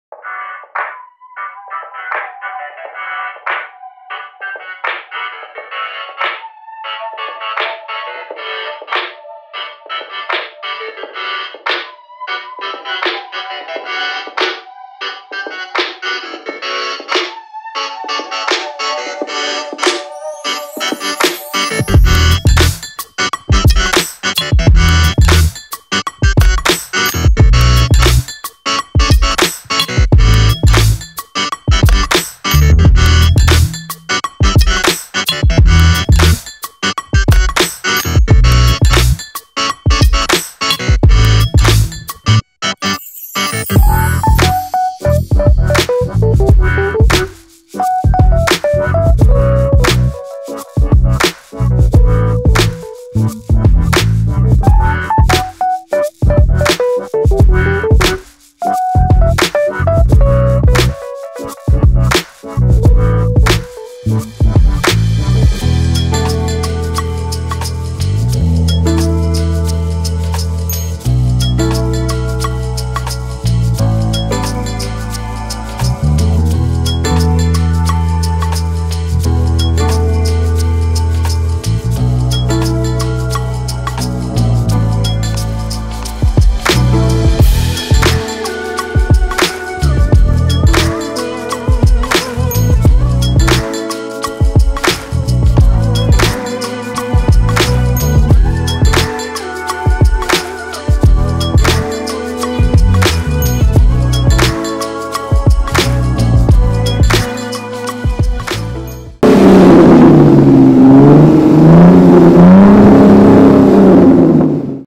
2 Neufahrzeuge - Volvo FH4 - Heisel & Heiner
Zwei Volvo Trucks FH4 Neufahrzeuge für die Borchers Transportlogistik NordWest GmbH.